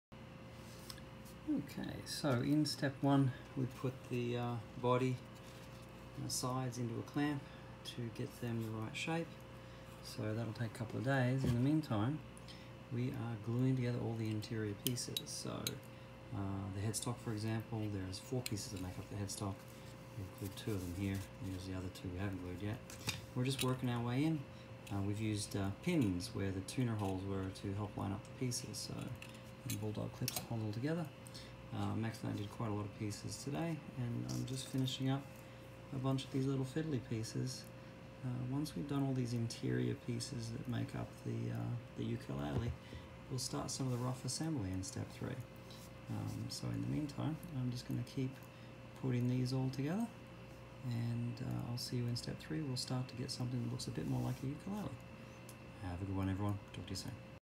Ukulele Build - Step 2 - Interior Parts
Ukulele build from scratch with Max.  Step 2 - glue up interior parts.

Want to build your own ukulele from laser cut parts?  You can purchase my soprano ukulele design files on Etsy